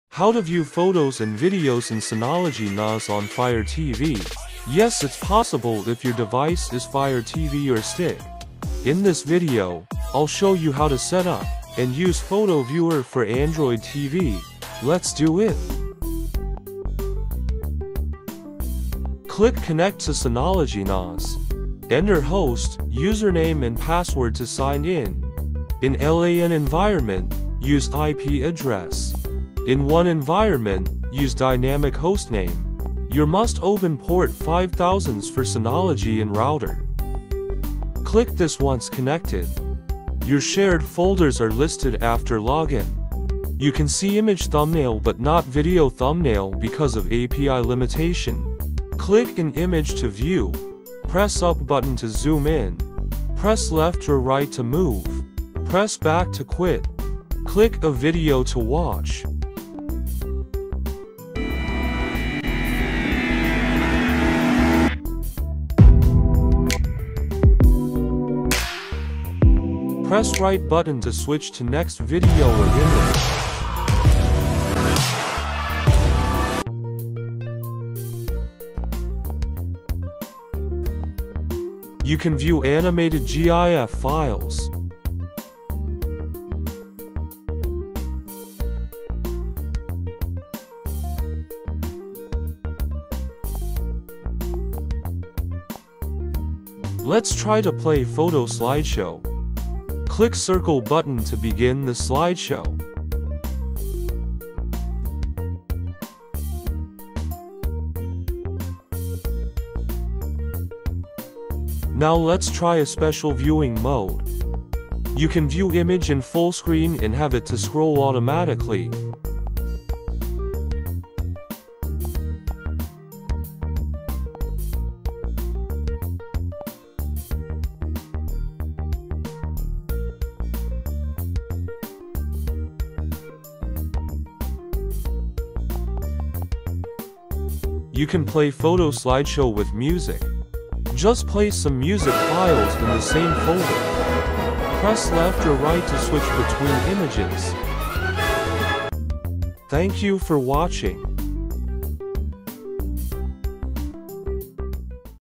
Fire TV & Firestick photo slideshow viewer from Synology NAS, USB, Google Photos
Can you access media files like photos and videos stored in Synology NAS on Amazon Fire TV cube or Fire TV stick with your remote control?
Yes it's totally possible, by using Photo Viewer for Android TV, it has direct support for Synology NAS API so no matter your TV is in LAN or WAN environment, you can access your Synology NAS.